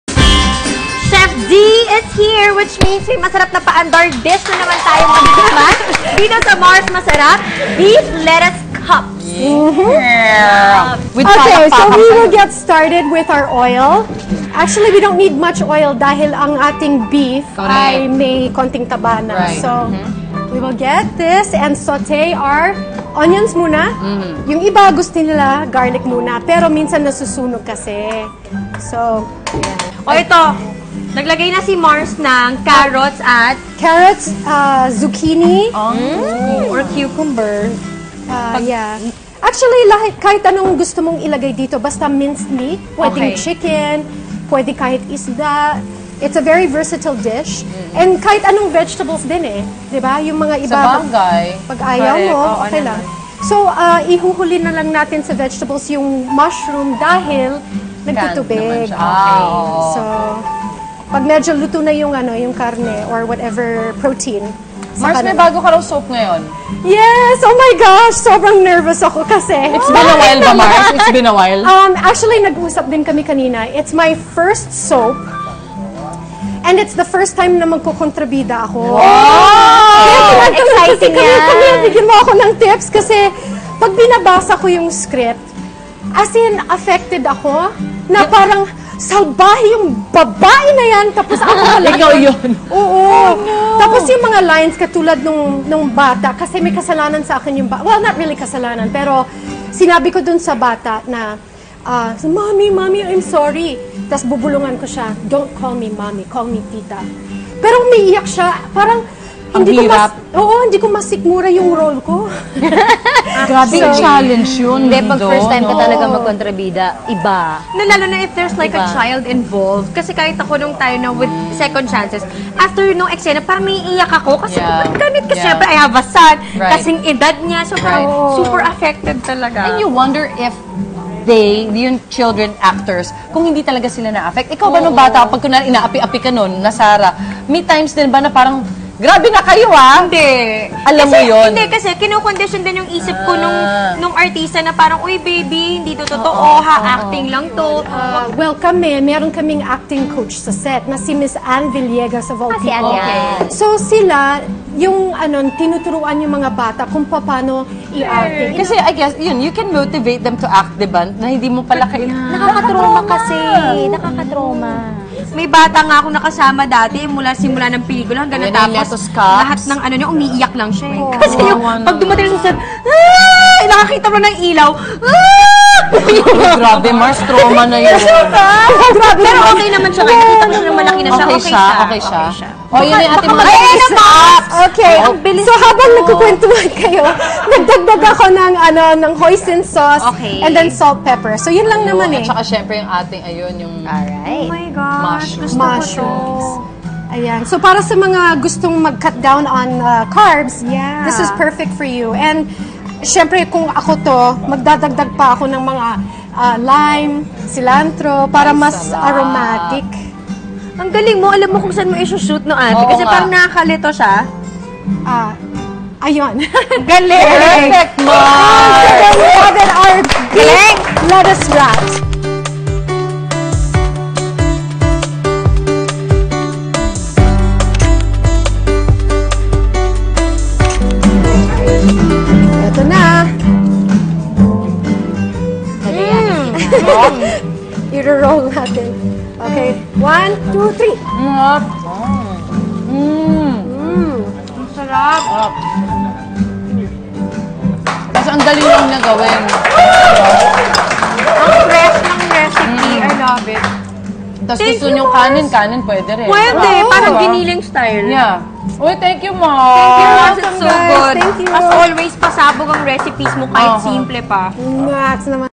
Chef D's Beef Lettuce Cups
Pasabog ang recipe ni Donita Rose habang nagkukuwentuhan ang mga mars tungkol sa kanilang showbiz experiences.